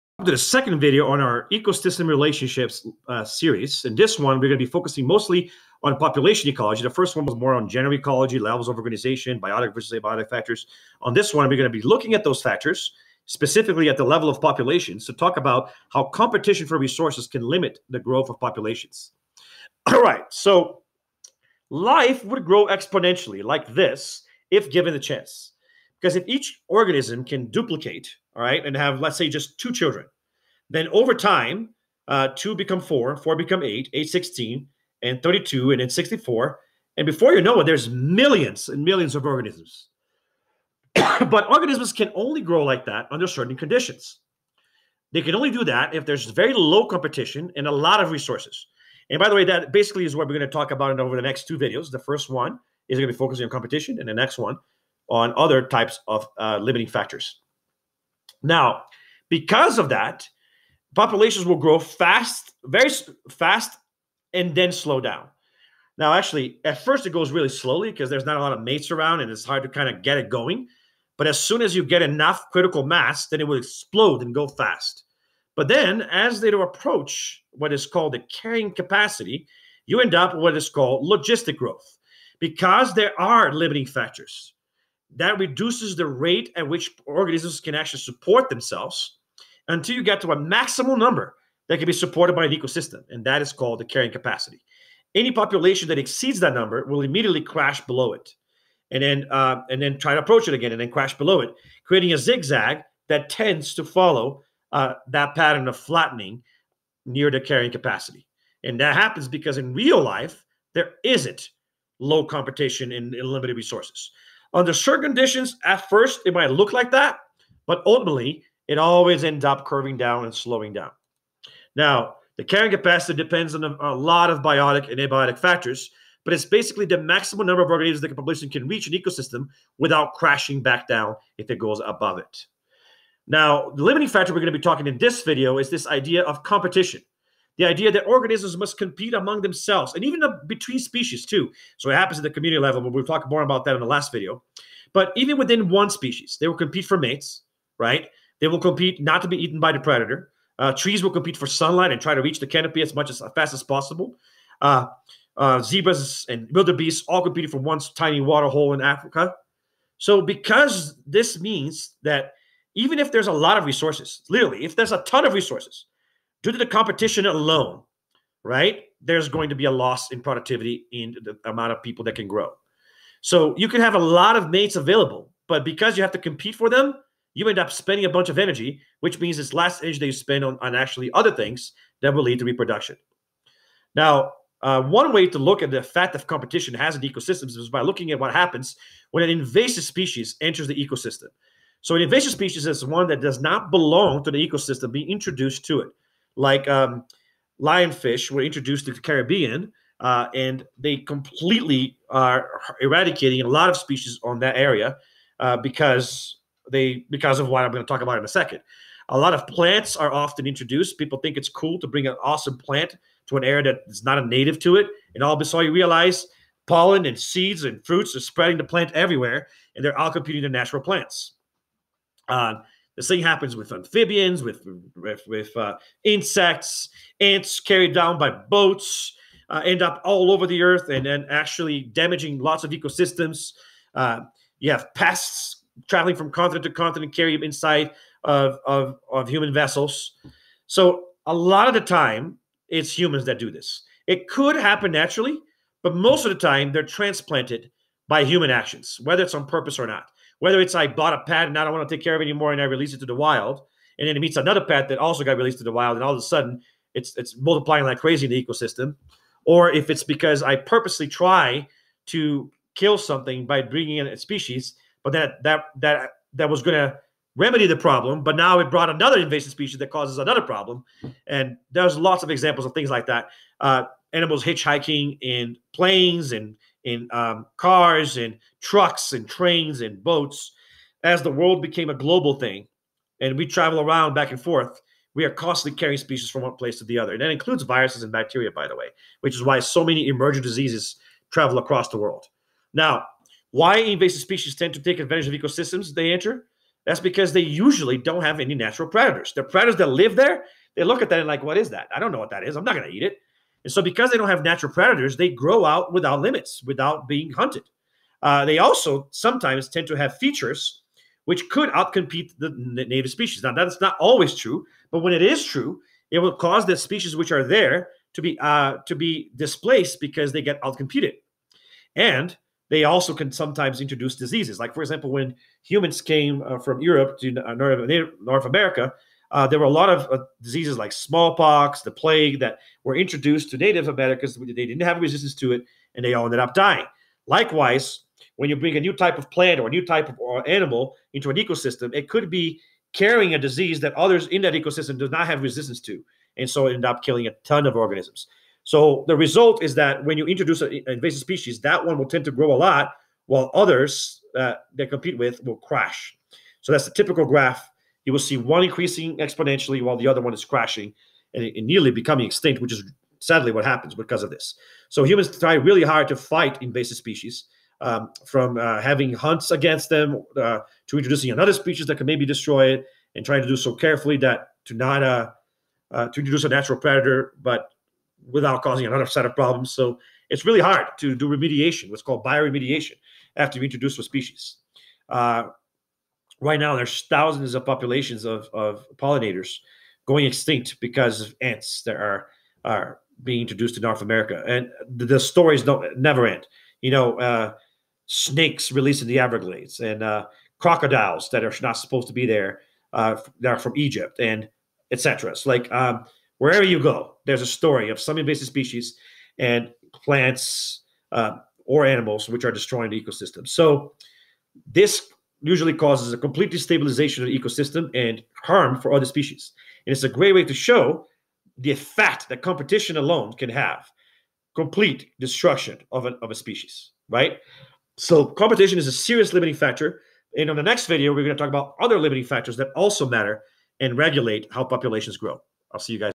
Ecosystems: Relationships - Objective 2: Population Ecology & Competition
Mr. Lima discusses the 3rd objective of the Ecosystem: Relationships content assessment. Click on SHOW MORE to see links to parts of the video.

Exponential vs. Logistic Growth (0:19) and the Carrying Capacity (1:23)
Competition (2:13)
Invasive Species (3:03)